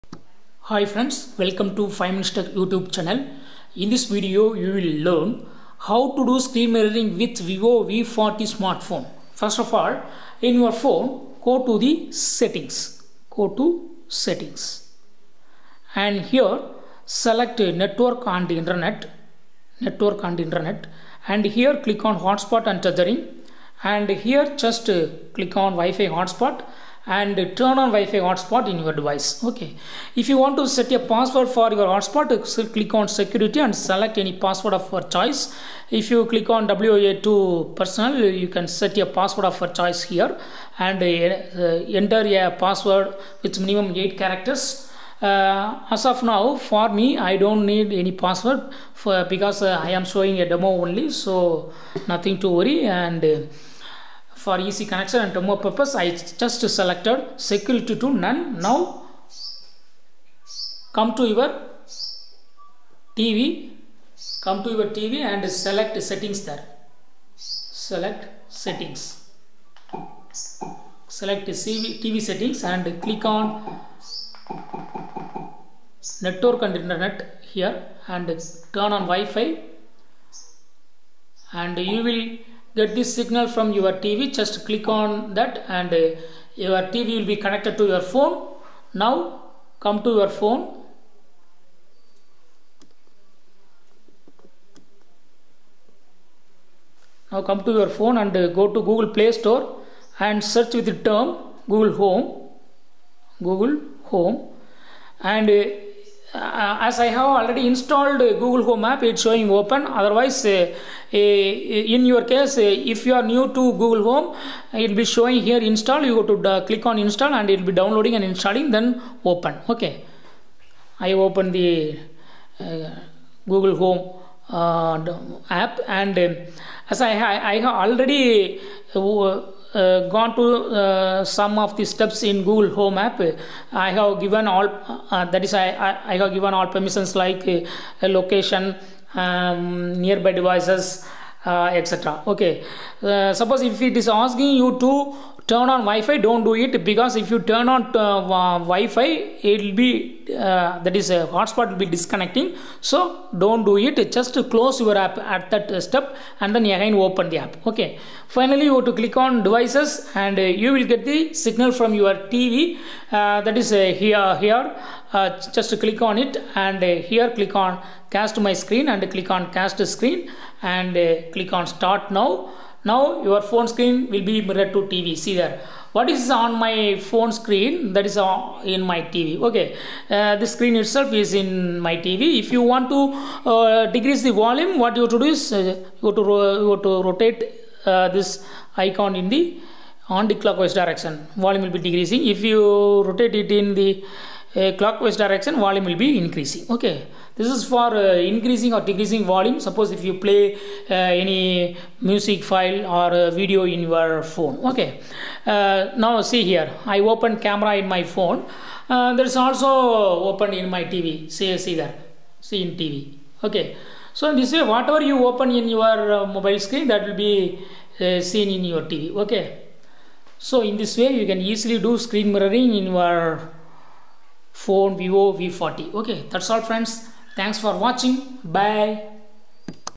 How to do screen mirroring in Vivo V40
Vivo V40 screen mirroring: Learn here how to do screen mirroring in Vivo V40. Screen mirroring is easier after watching this video of how to do screen mirroring in Vivo V40.